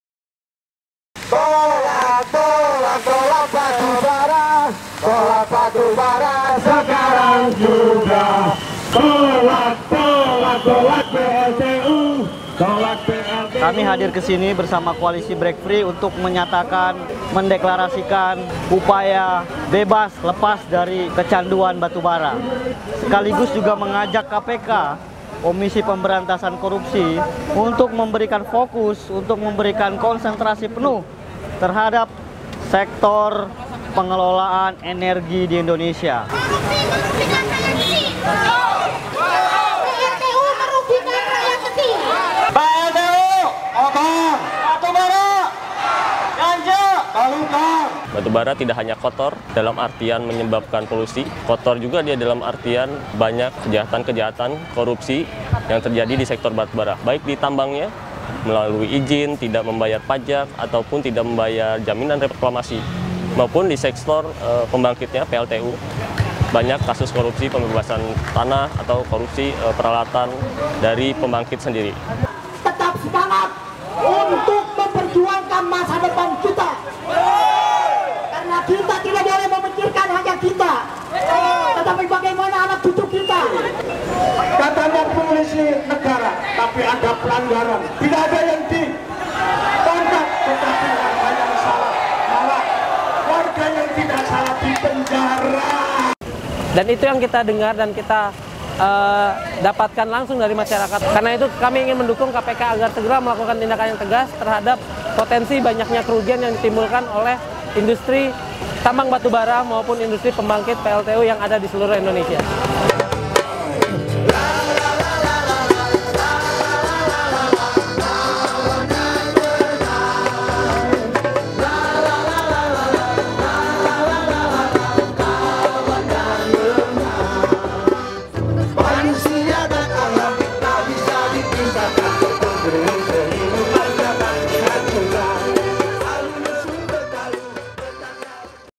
Break Free 2017 - Jakarta
23 Maret 2017, koalisi Break Free bersama masyarakat korban tambang, batu bara dan pembangunan PLTU dari berbagai daerah di Indonesia berjalan bersama mendatangi KPK dan Istana Presiden untuk mendukung pengungkapan korupsi di sektor tambang, batu bara dan PLTU batubara serta mendukung pemerintah untuk beralih ke energi terbarukan.